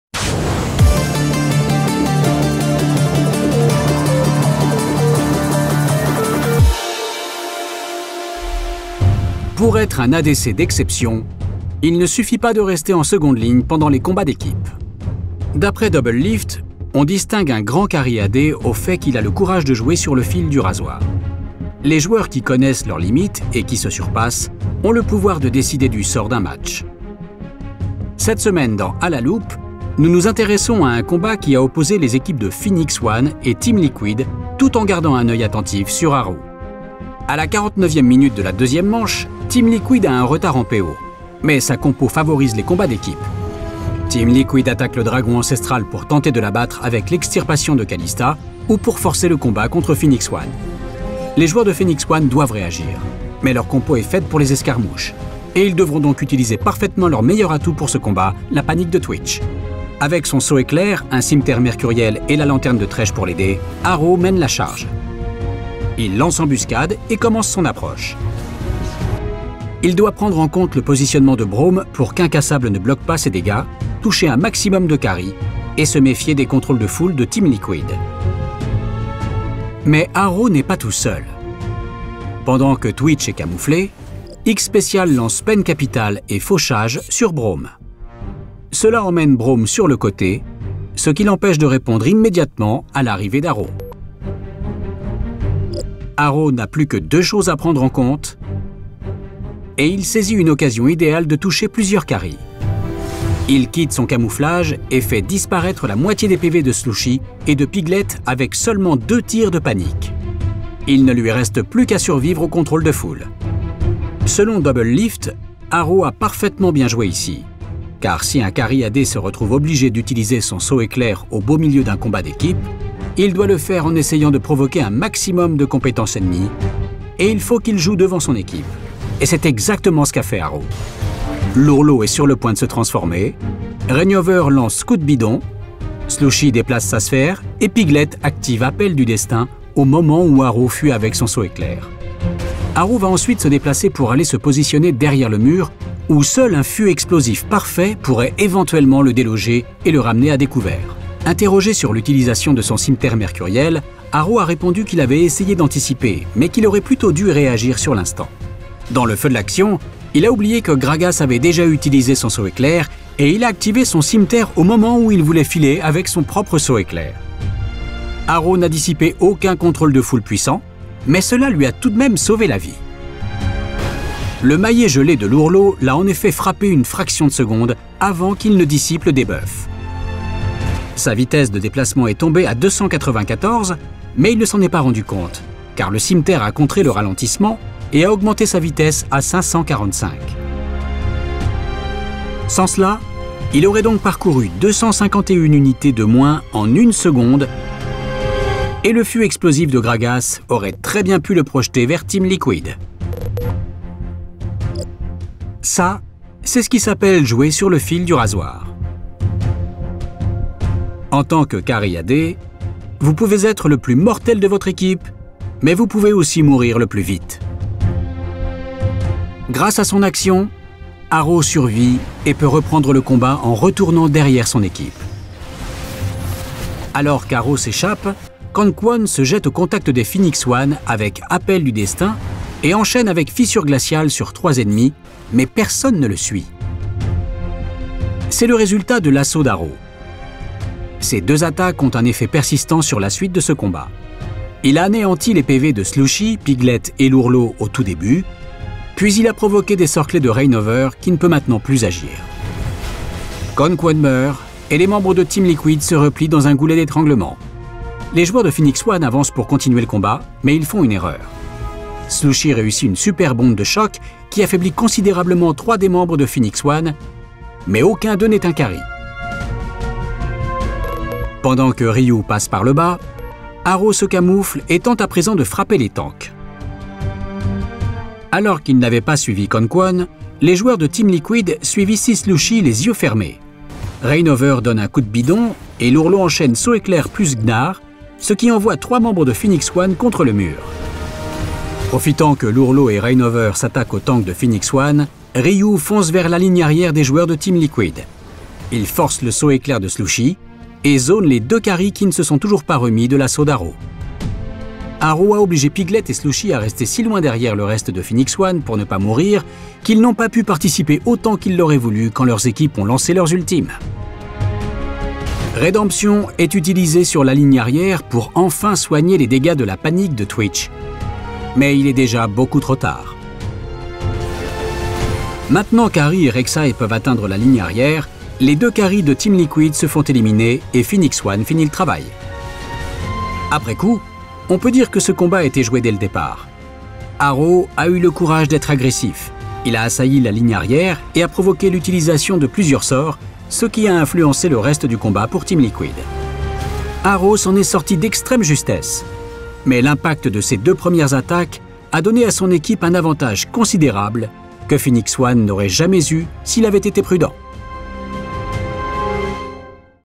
À La Loupe : Épisode #6
Cette semaine dans « À La Loupe », Nous nous intéressons à un combat qui a opposé les équipes de Phoenix 1 et Team Liquid lors des LCS NA.